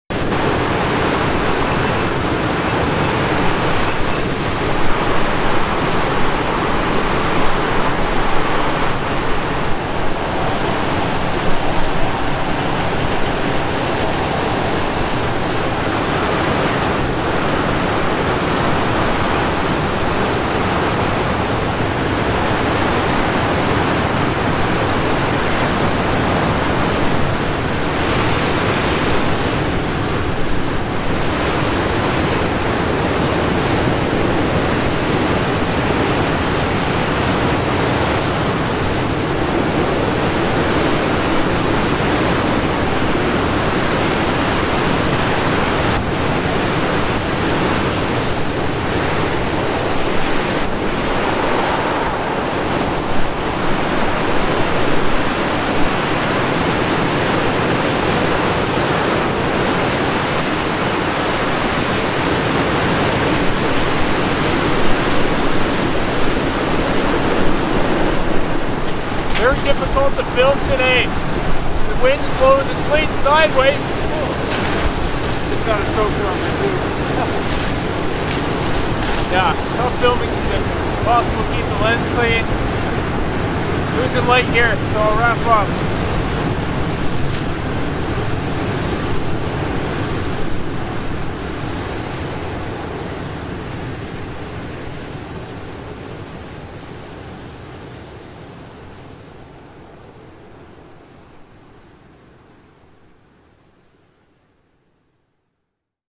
Violent Fall Storm on Lake Superior (4K)
Violent Storm on Lake Superior - 12/16/2021 (4K)

⮞⮞⮞ Support Lost Lakes by using these Amazon links if you're interested in the gear we use⮜⮜⮜
⮞  CANADIAN 🍁 (.ca) LINKS:  ⮜